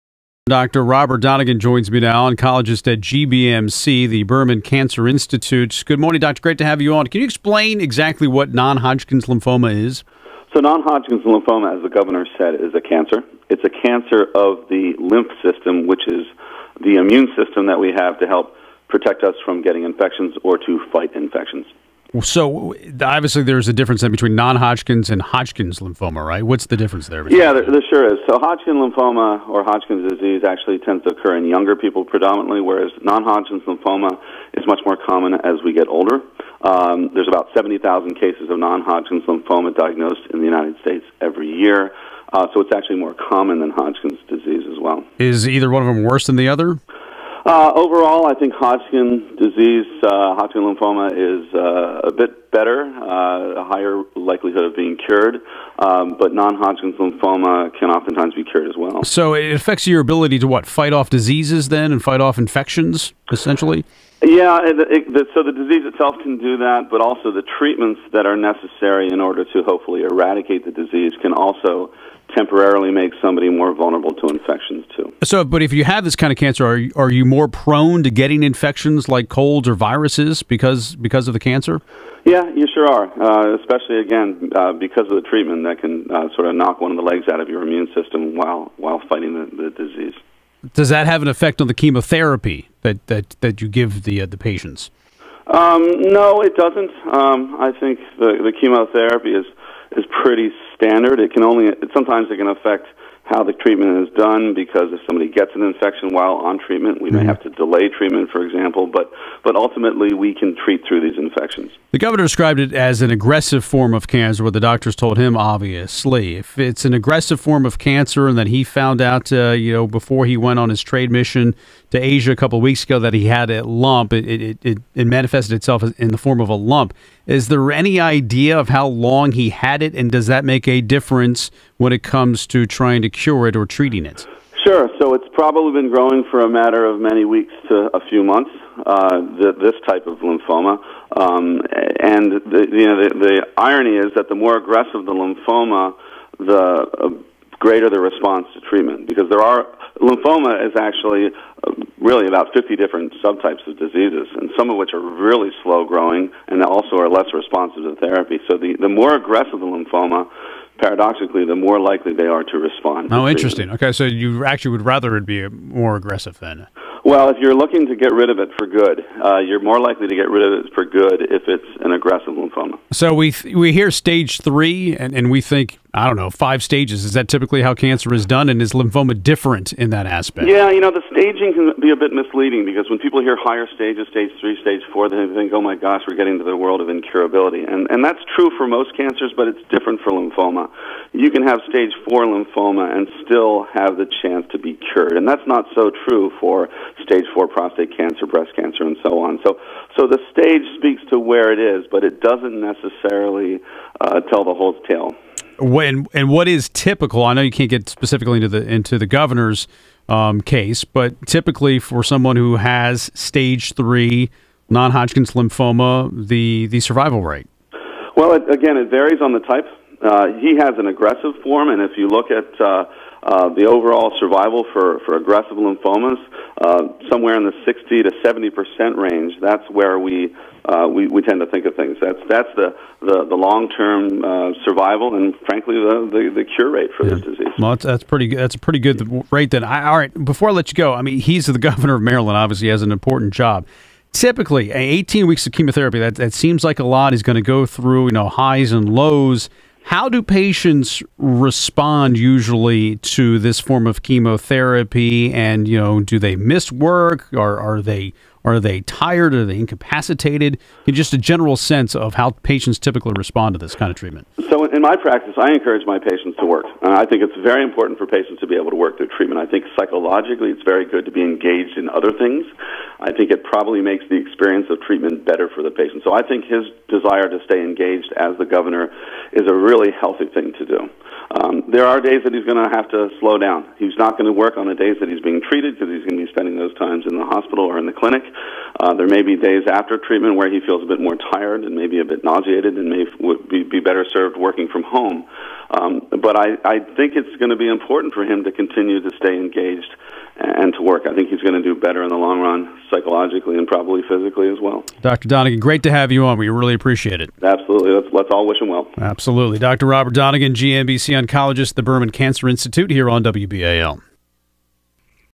"What is Lymphoma?" - Dr. Donegan on WBAL Radio
GBMC oncologist Dr. Robert Donegan of The Sandra & Malcolm Berman Cancer Institute spoke with WBAL 1090 AM Radio this morning about what Non-Hodgkin's lymphoma is, what the treatment involves and what it means to have an "aggressive" form of lymphoma. GBMC wishes Gov. Larry Hogan well as he begins his treatment.